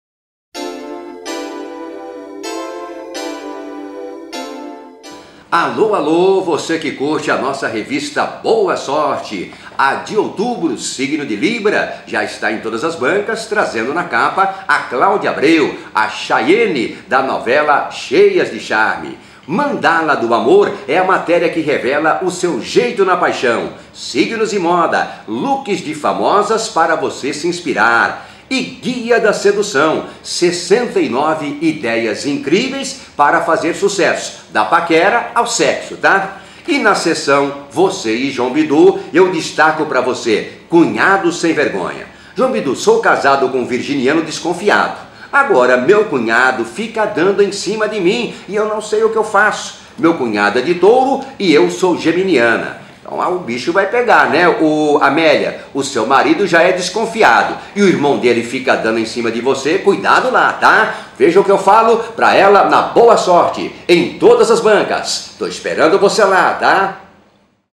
Boa Sorte - outubro de 2012 - "Meu cunhado fica dando em cima de mim"
Confira na Boa Sorte de outubro:

- Claudia Abreu, a "Chayene" da novela "Cheias de Charme", está na capa!
- Descubra o seu jeito na paixão com a Mandala do Amor!
- Signos e moda: looks de famosas para você se inspirar;
- Guia de sedução: 69 ideias incríveis para fazer sucesso da paquera ao sexo;
- "Cunhado sem vergonha: ele fica dando em cima de mim. O que eu faço, JB?"
- Previsões para outubro

Garanta já a sua!